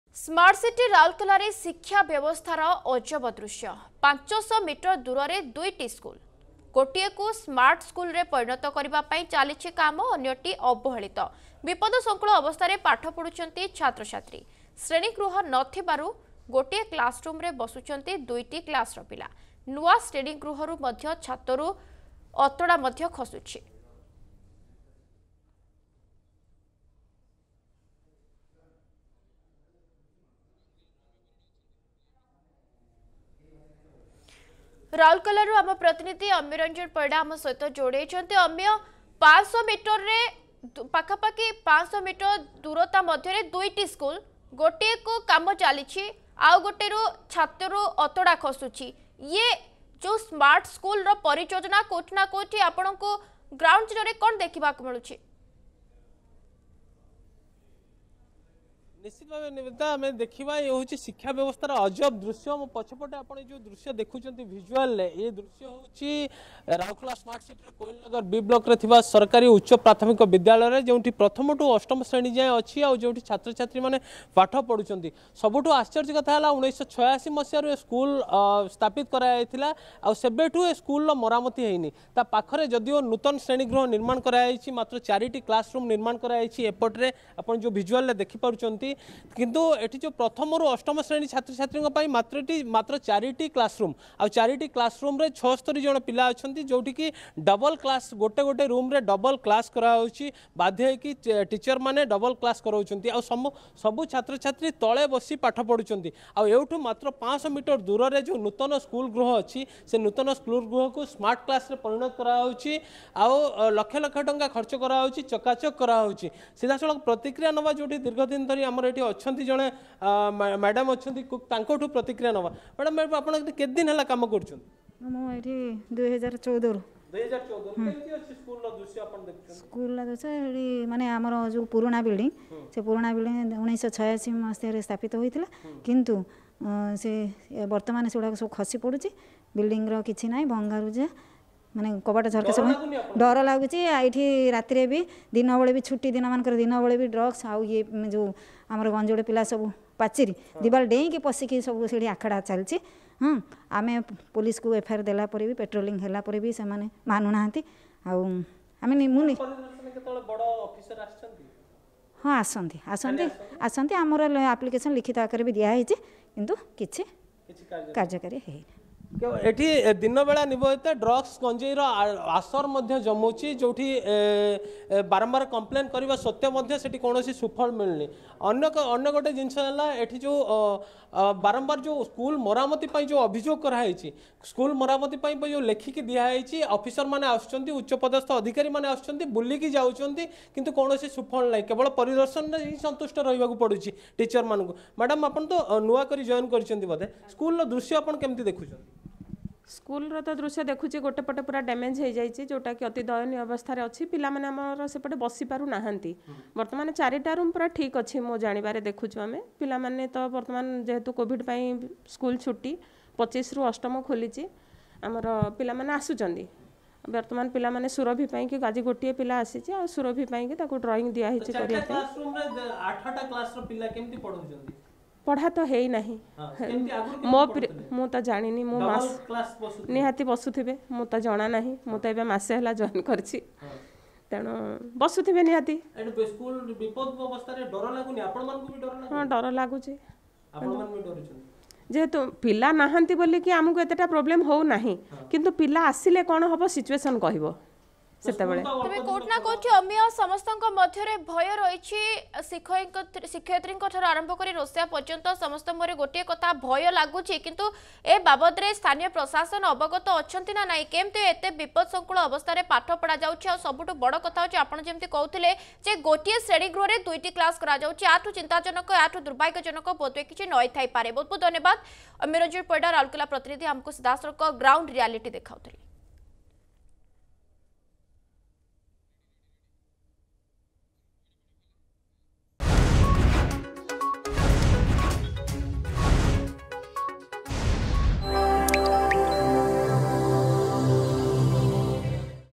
ରାଉରକେଲାରେ ଶିକ୍ଷା ବ୍ୟବସ୍ଥାର ଅଜବ ଦୃଶ୍ୟ
ସ୍ମାର୍ଟ ସିଟି ରାଉରକେଲାରେ ଶିକ୍ଷା ବ୍ୟବସ୍ଥାର ଅଜବ ଦୃଶ୍ୟ। 500 ମିଟର ଦୂରରେ ଦୁଇ ଟି ସ୍କୁଲ । ଗୋଟିଏ କୁ ସ୍ମାର୍ଟ ସ୍କୁଲ ରେ ପରିଣତ କରିବା ପାଇଁ ଚାଲିଛି କାମ ଅନ୍ୟଟି ଅବହେଳିତ । ବିପଦ ସଂକୁଳ ଅବସ୍ଥାରେ ପାଠ ପଢୁଛନ୍ତି ଛତ୍ର ଛାତ୍ରୀ।